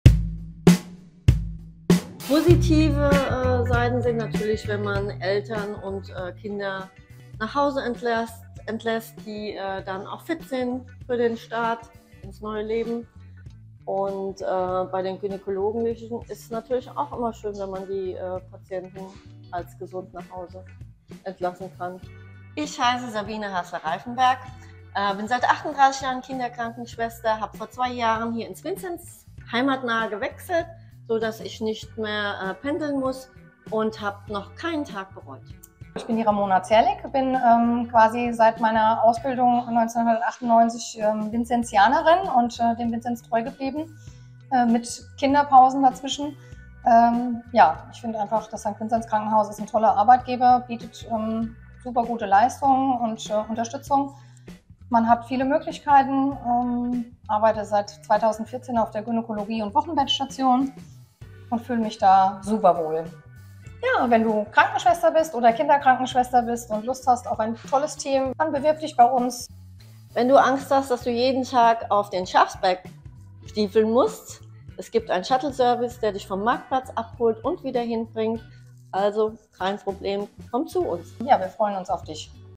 Pflege im Vincenz: Pflege mit Teamgeist
Die eine ist Vincenzianerin von Anfang an, die andere ist noch relativ frisch dabei - doch ganz egal: Ramona Zerlik und Sabine Haasler-Reifenberg sind nicht nur Kolleginnen, sondern auch Freundinnen.

Neben der Freude, junge Familien beim Start ins Leben zu begleiten, ist für sie ganz klar ihr Team DER Grund, warum sie jeden Tag gerne zur Arbeit gehen.

Welche Benefits sie im St. Vincenz besonders schätzen und warum neue Kolleg:innen keine Angst vor der "Besteigung des Schafsbergs" haben müssen, erklären sie im Video.

Erfahre mehr über den Pflegeberuf im St. Vincenz-Krankenhaus und bewirb Dich am besten direkt über unser Bewerbungsformular